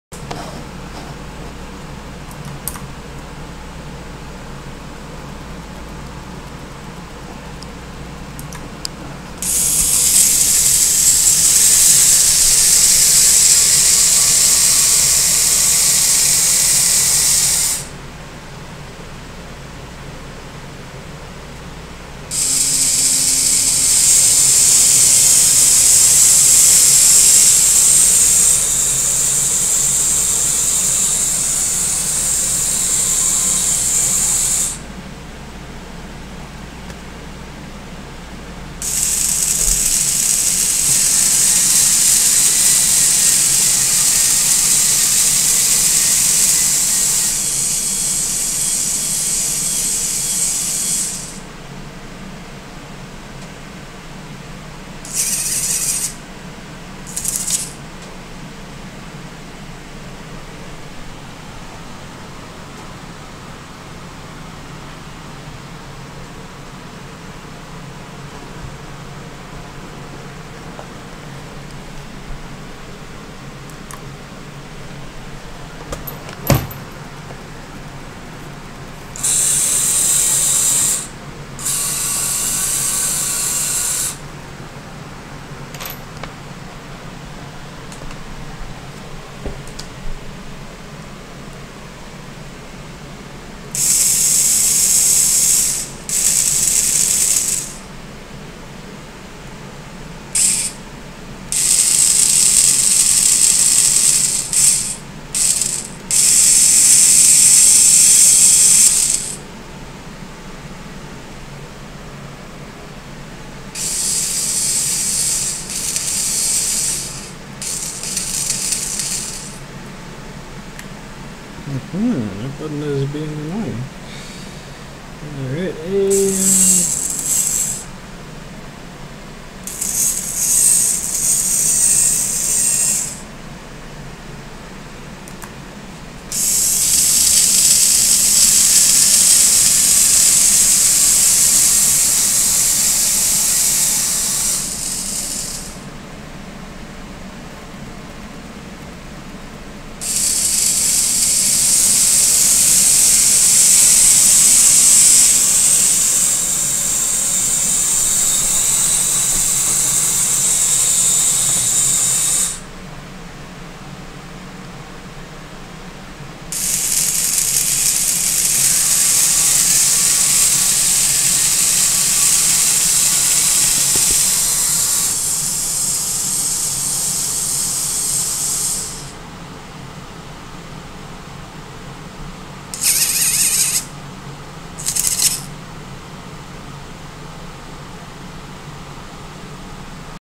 VEX Robotics 5 Axis + Linear Slide Base Robot Arm Using Joint Move!
Holy heck! It's been quite a ride this past week. I've been working hard all week to get this robot into a presentable condition for club expo day at Hennepin Technical College. I finally got rid of the integrated motor encoder on the wrist roll and replaced it with a pot.

My code uses a fairly cool system whereby points are made up of a typedef struct containing variables for all the joint degree values, and through a clever trick, I can get all these motors running to the correct positions. My next step is to work in my magical trigonomegnomes to get cartesian coordinates working. Then I can make it do something "useful".